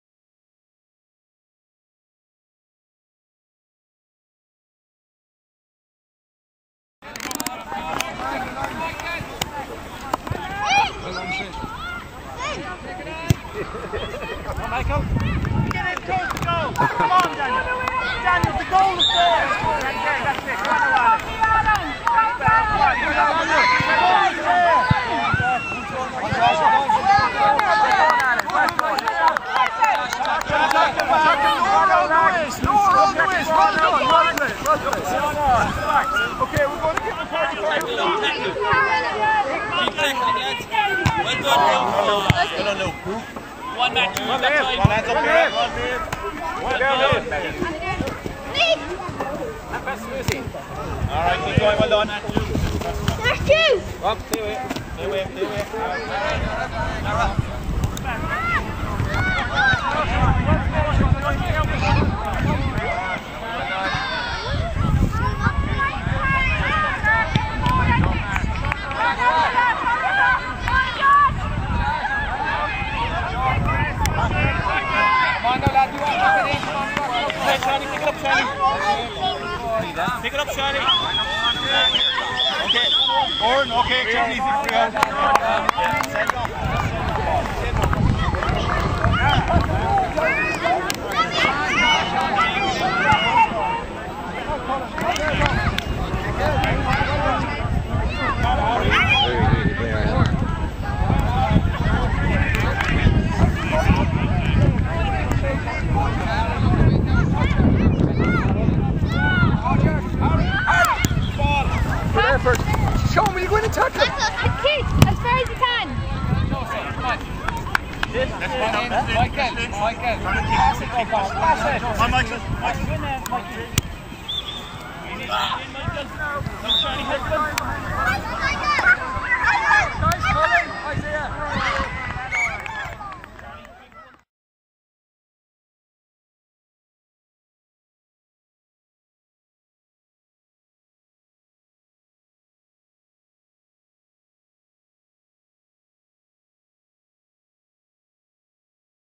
Cork GDA U7 & U8 Football Blitz Aghada 16th Aug 2014
U7 & U8 Football Blitz in Aghada GAA pitch on Sat 16th Aug 2014 organised by Cork GDA Sean Crowley, Film Shane Supple